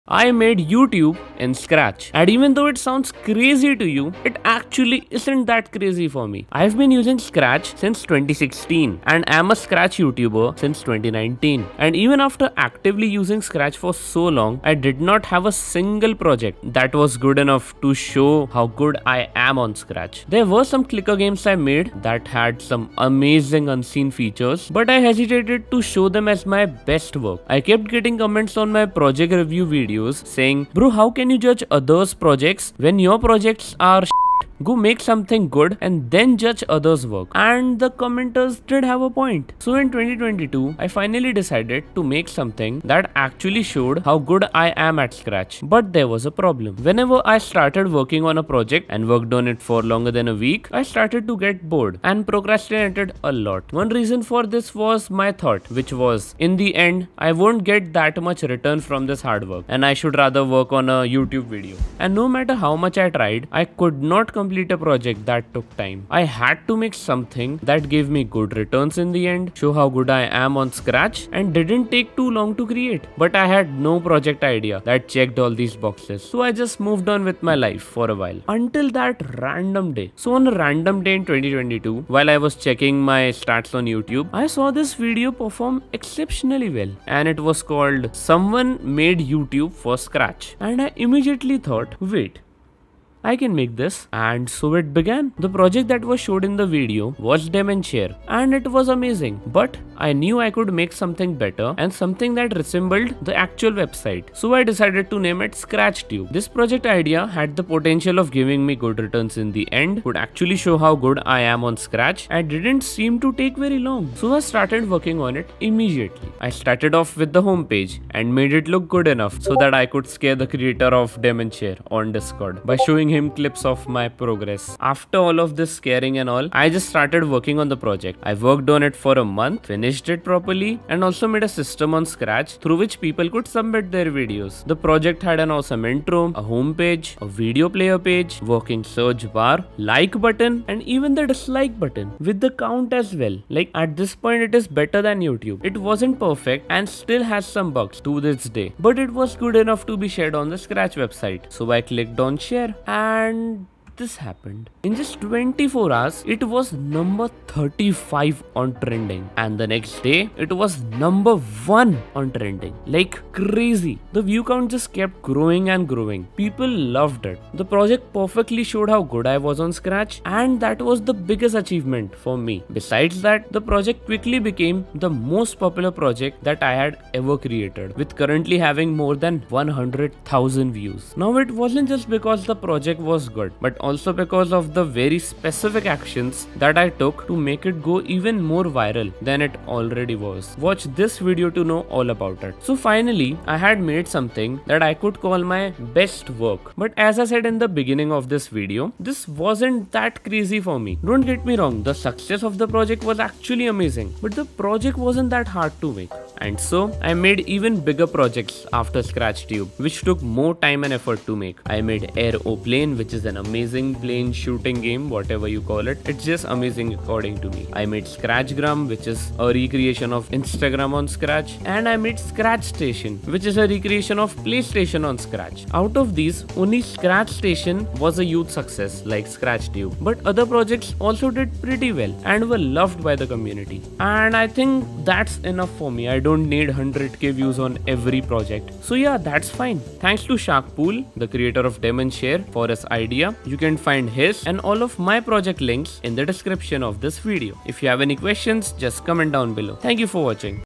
I made YouTube in Scratch!
In this video, I'll share the time when I made YouTube in Scratch!

😻 DemonShare (This is not the Original as the Original got taken down)

▶️ Social Links!

⏱️Timestamps/Video Chapters⏱️
0:00 Intro